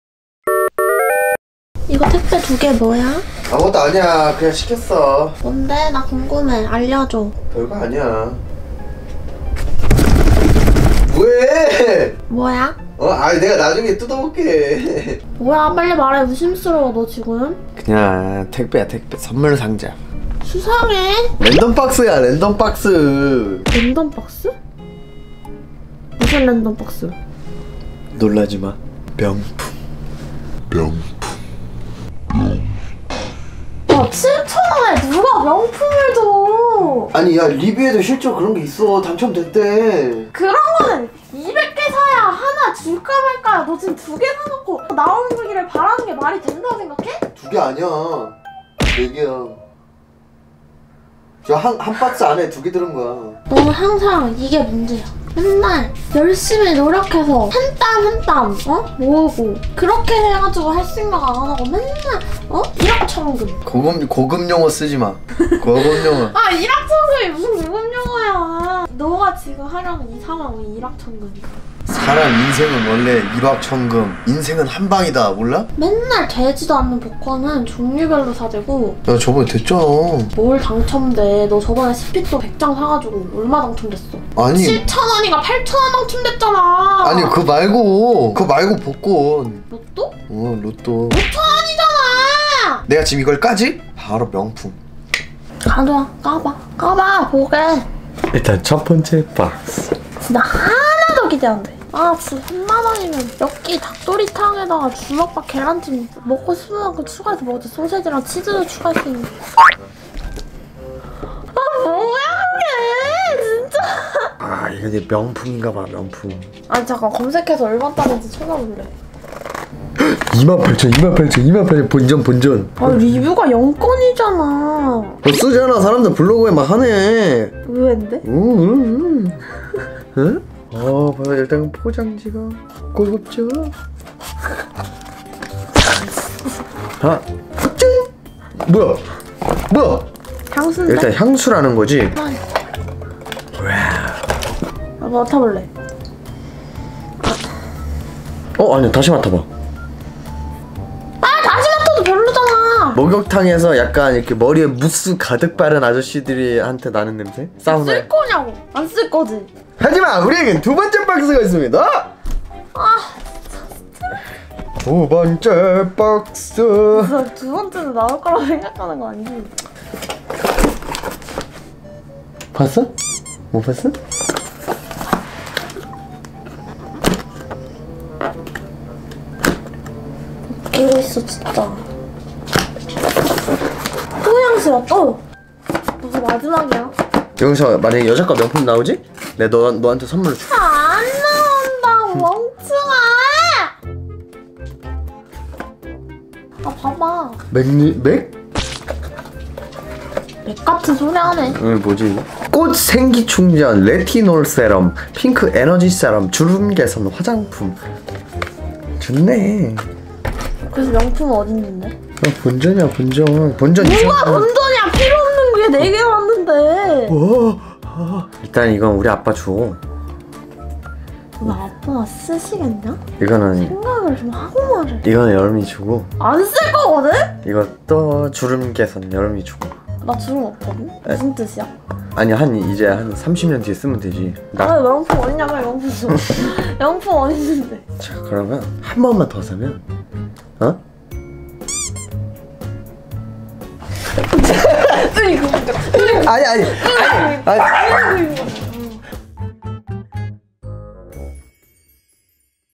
[석준이가해] 명품이 갖고 싶은 철없는 연하 남자친구ㅋㅋㅋㅋㅋㅋㅋㅋㅋㅋ
석준이가 갖고 싶었던 것 : 구찌 반지갑, 구찌 벨트, 구찌 신발
랜덤박스에서 나온 것 : 정체 모를 화장품, 아저씨 향수